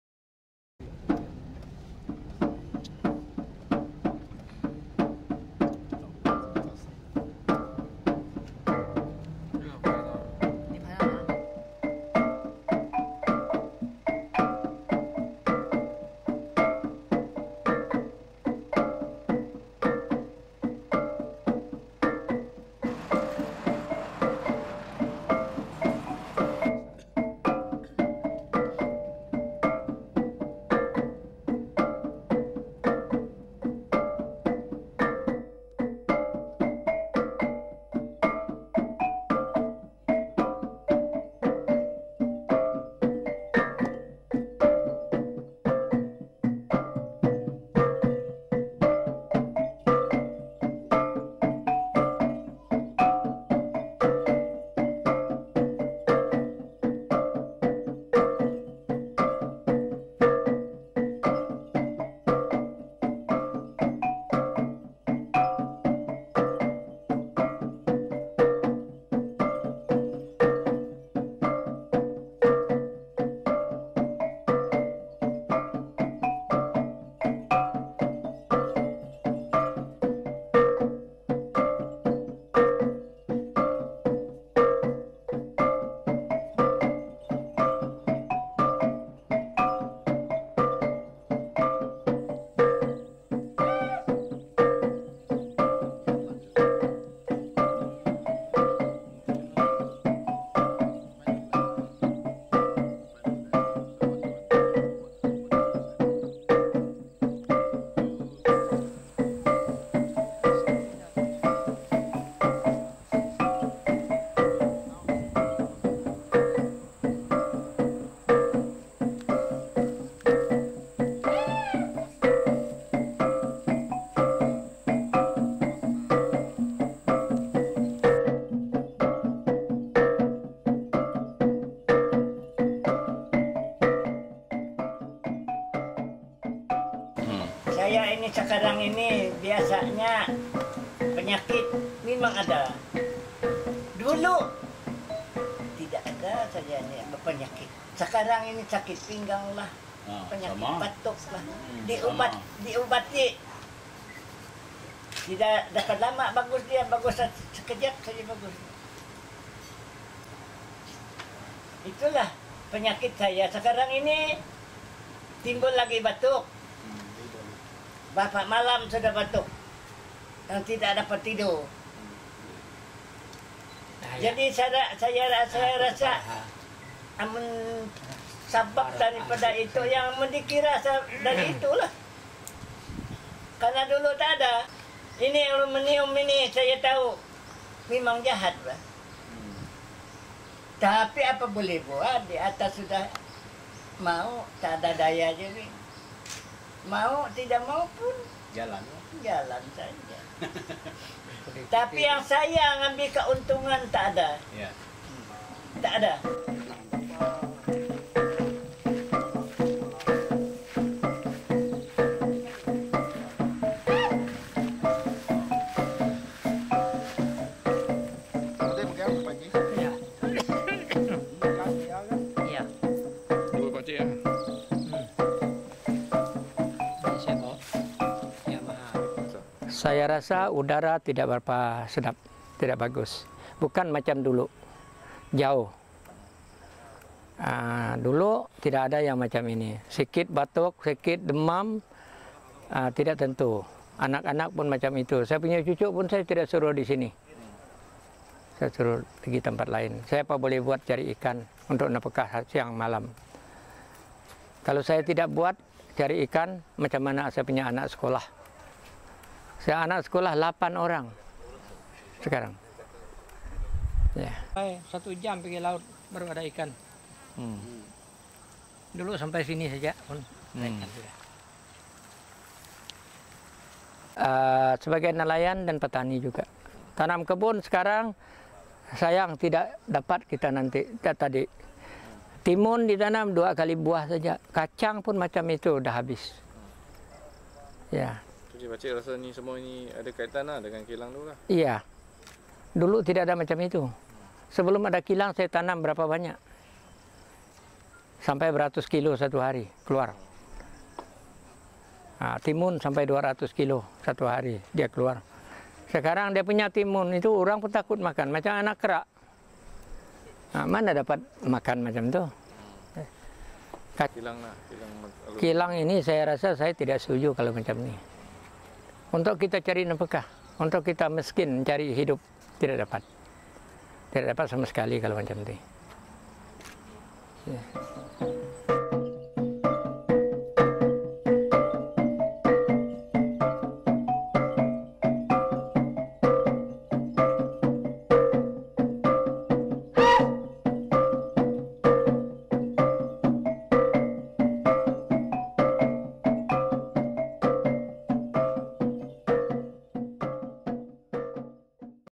什么烂叫炼铝厂 the New Aluminium Smelting Plant in Similajau
A new Aluminium Smelting Plant own by Press Metal Sarawak（Malaysia）had started to operate for more than 3 months in Samalaju Industrial Park,Bintulu under SCORE project. Our visit revealed that villagers at Kampong Nyalau Ulu and fishermen living adjacent to the area complain of respiratory problems and sea water pollution.

由齐力炼铝厂拥有的第二间炼铝厂已悄然的在砂拉越Samalaju工业区操作了三个多月，我们的考察显示当地居民，农民和渔民已开始投诉遭遇各种和呼吸器官相关的症状，海水污染和农作物的枯死等问题。